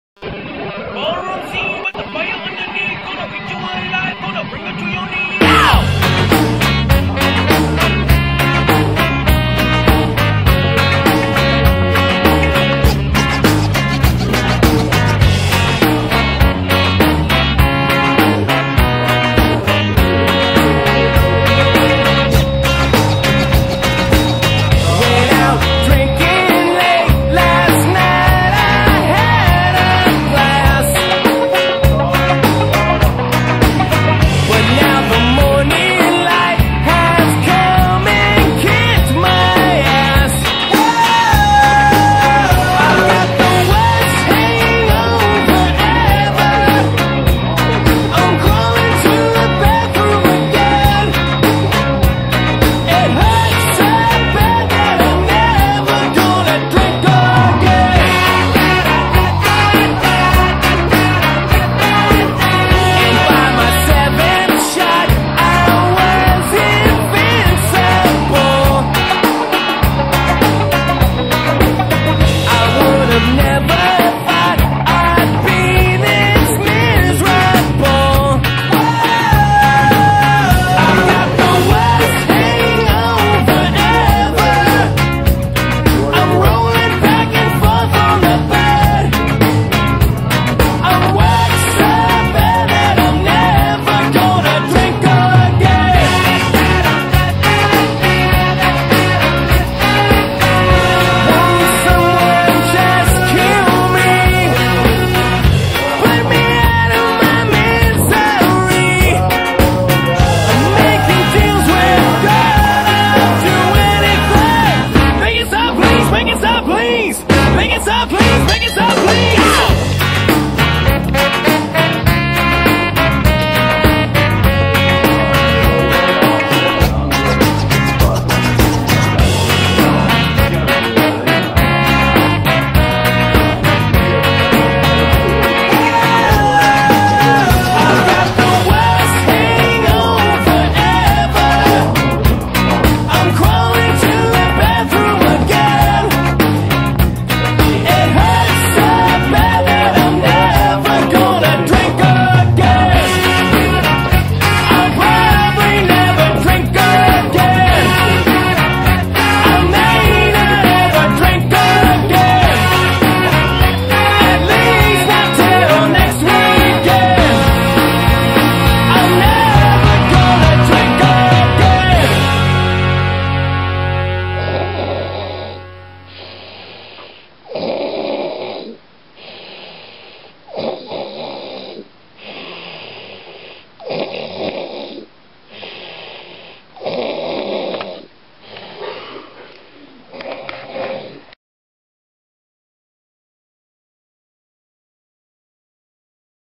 Drunk Braz part 2
more scenes from his drunken night with the vacuum